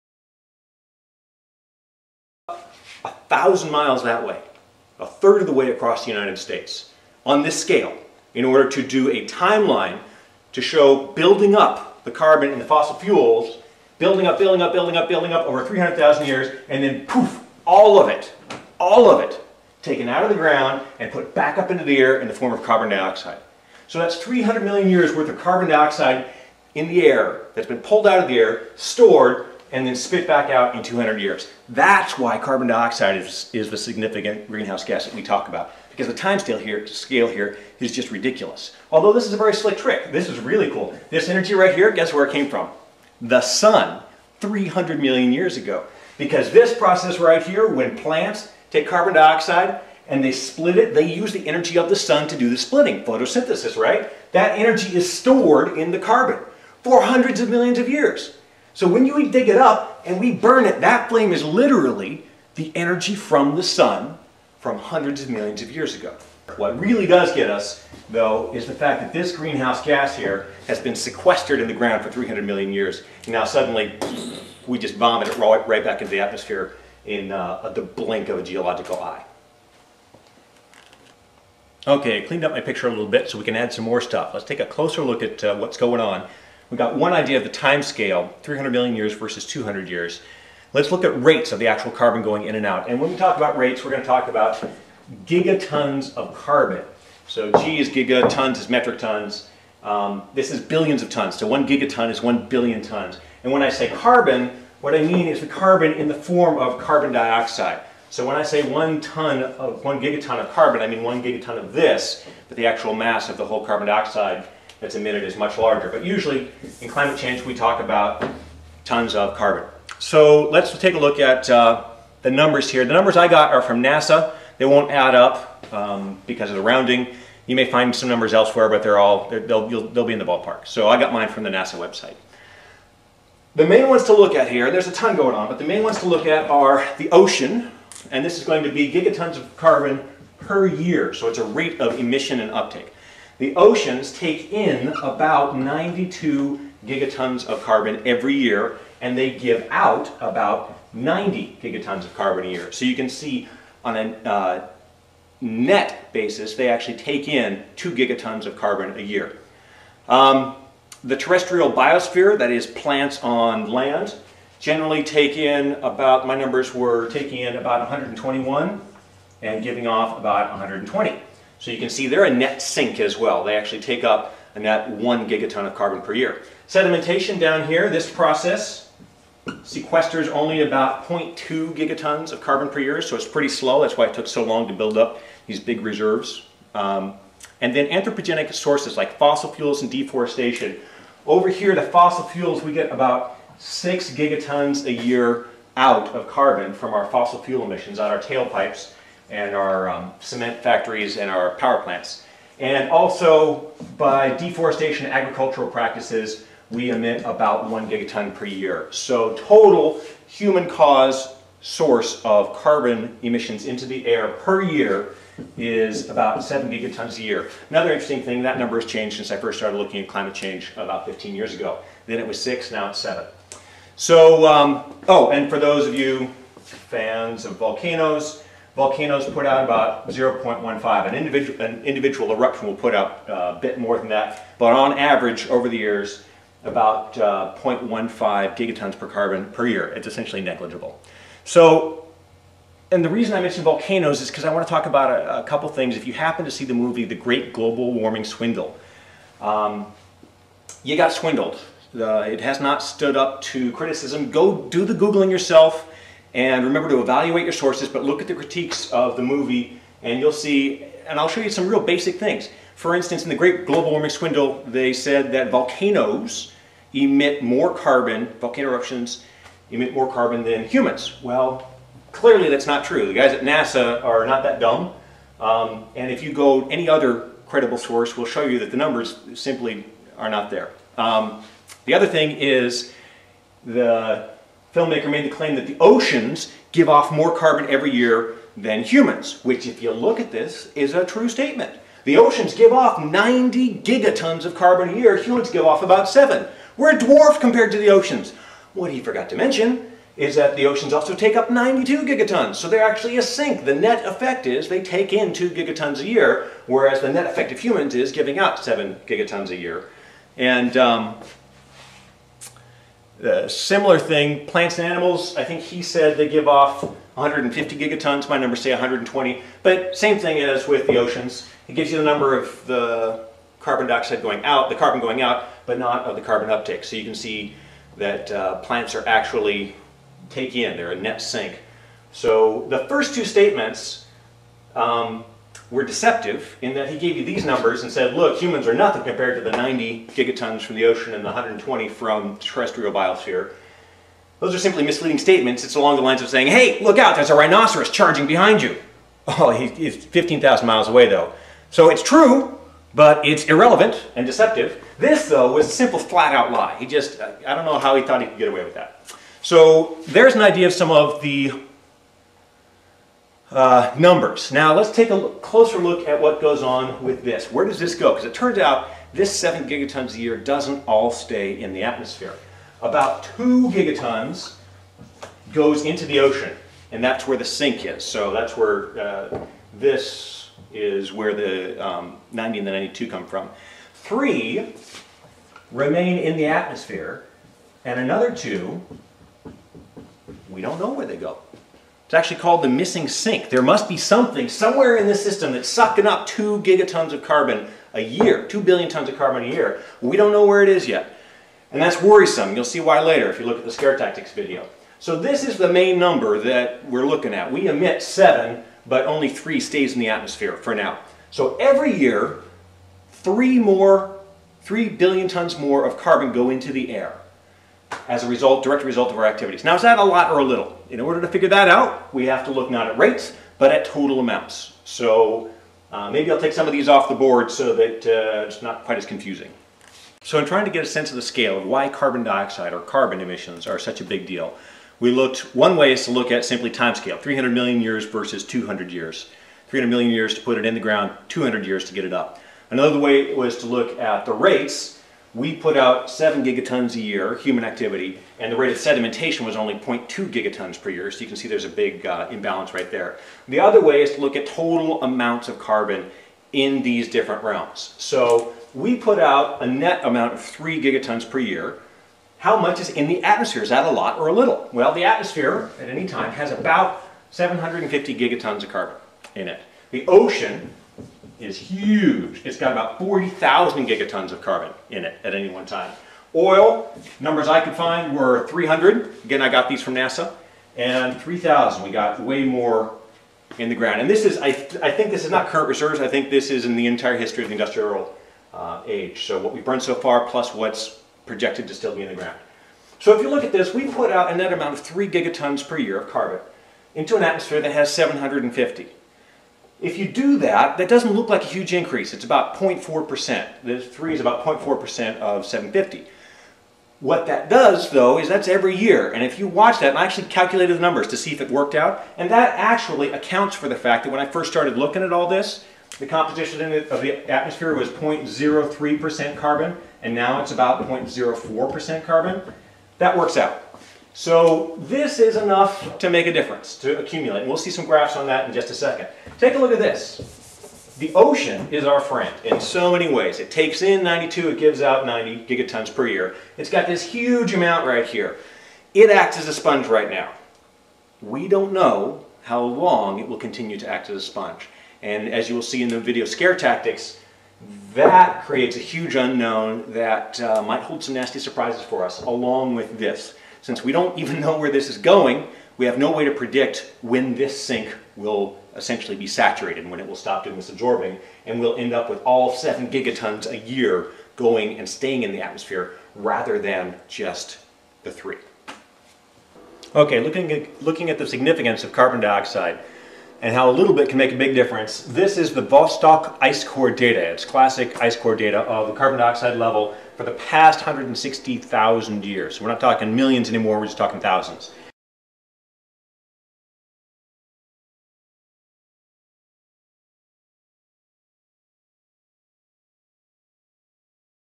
How It All Ends: Mechanics of GCC (pt 2 of 3)
OOOPS! (4/4/08) Commentors guffpot and DavidSChandler caught a couple errors I made during my RedBull-fueled exposition. guffpot correctly pointed out that I compared 200 years vs. 300 million years incorrectly. Those two numbers are apples and oranges, because the carbon we transfer from the ground to the air in 200 years was not stored over a period of 300 million years--it was stored FOR 300 million years, and but was actively sequestered over a time period probably much shorter. Really, it would be more proper to compare the 200 years it's taking us to transfer from the ground to the air to the time period it took to transfer it from the air to the ground. If anyone can find a credible figure for that, please message me directly, and I'll include it here.

DavidSChandler caught that I was off by a factor of 100 in my scale model comparison of 200 years to 300 million years. If my line on the board is 200 years, then 300 million years ago would be about 10 miles--not 1000 miles--to the left. Turns out I forgot to convert from cm to m in my quick calculation. My bad. It's especially ironic because I'm constantly warning my students against making that same exact mistake! Ha! The joke's on me. I guess it just goes to show you--you can always be wrong. . .

Part of the "Expansion Pack" of videos accompanying the video "How It All Ends." See "How It All Ends: Index" for a roadmap to guide you through the expansion pack.